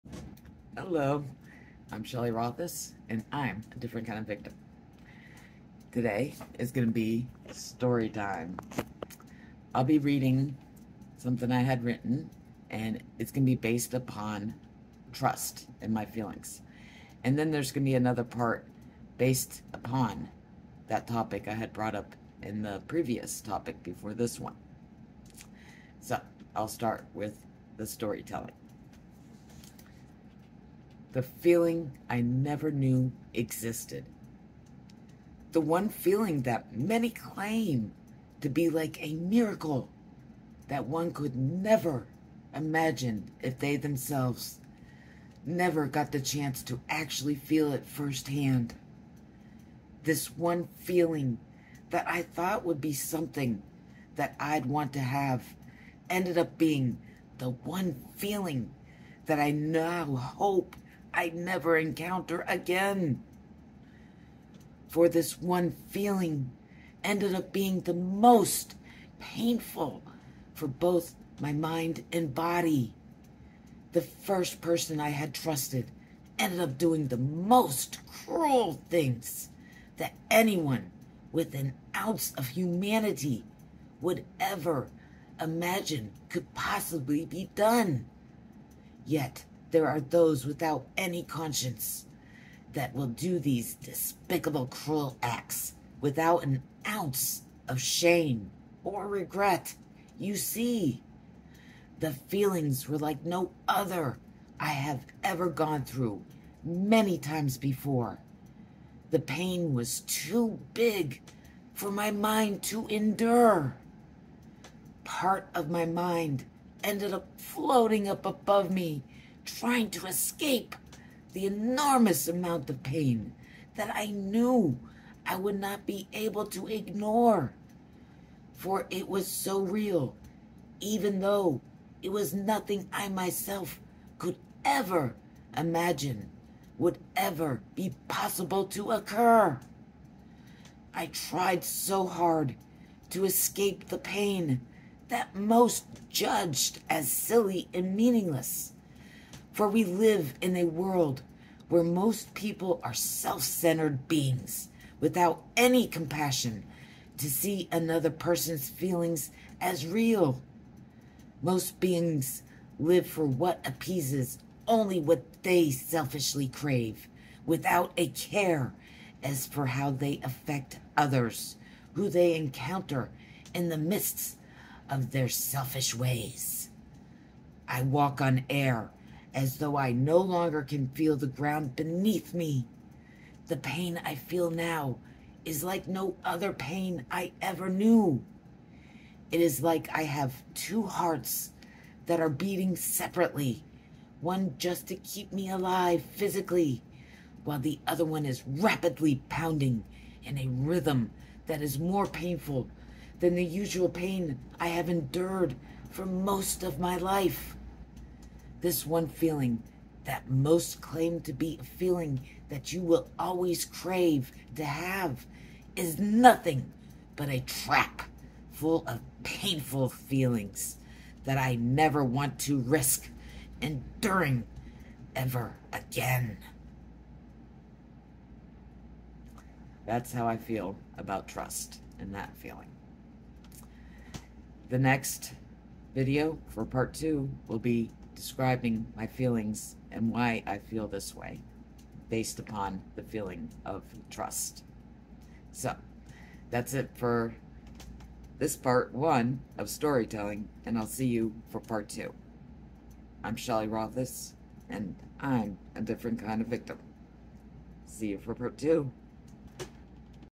Shelley Rothus Topic Part one: Story telling time. Trust
My feelings based upon how I felt after my trust was broken from the first person I trusted.